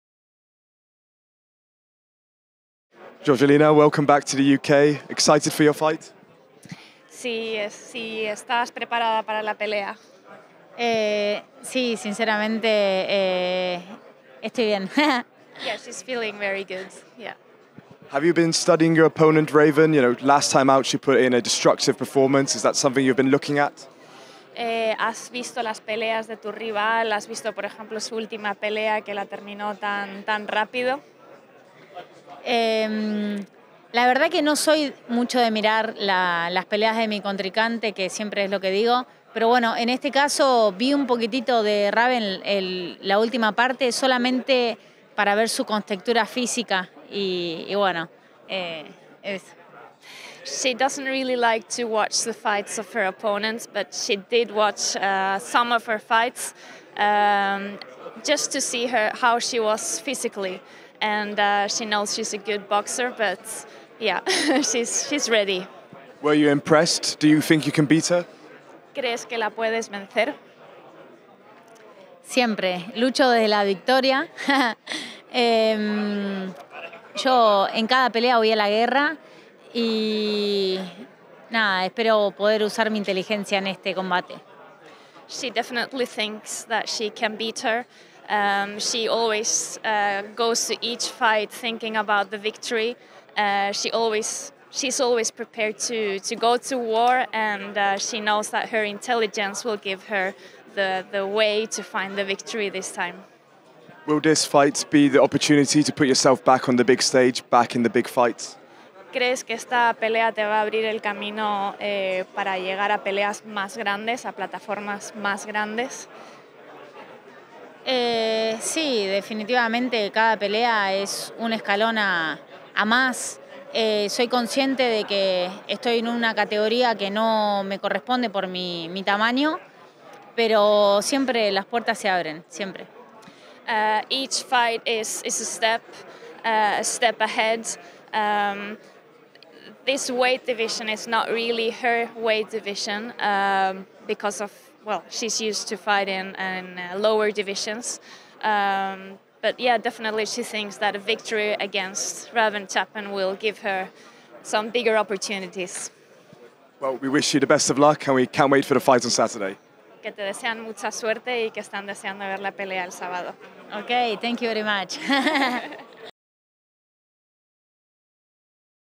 "I CAN BEAT HER!" | Jorgelina Guanini thinks she can shock Raven Chapman this weekend
We chat to Jorgelina Guanini ahead of her clash with Raven Chapman in Manchester on Saturday.